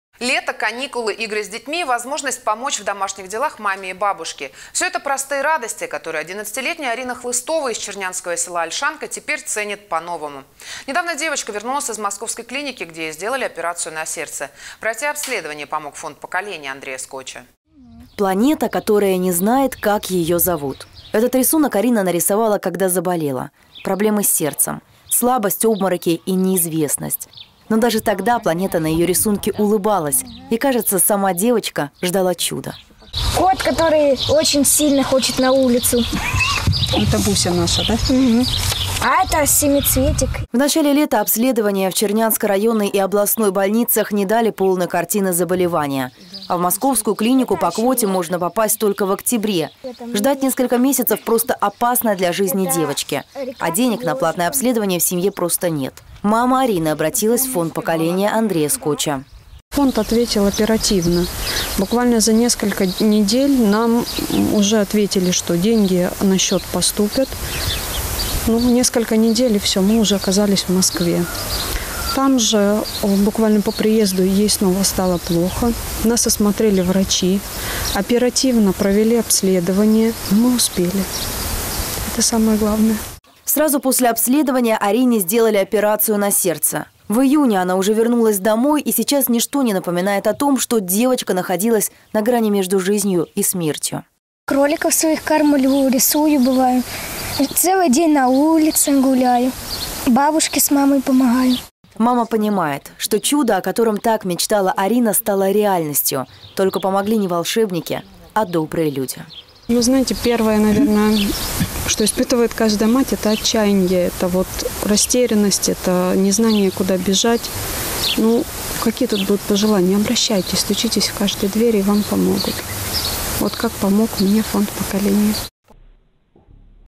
И пусть планета узнает, как ее зовут...
Лето, каникулы, игры с детьми и возможность помочь в домашних делах маме и бабушке - все это простые радости, которые 11-летняя Арина Хлыстова из чернянского села Ольшанка теперь ценит по-новому. Недавно девочка вернулась из московской клиники, где ей сделали операцию на сердце. Пройти обследование помог Фонд "Поколение" Андрея Скоча.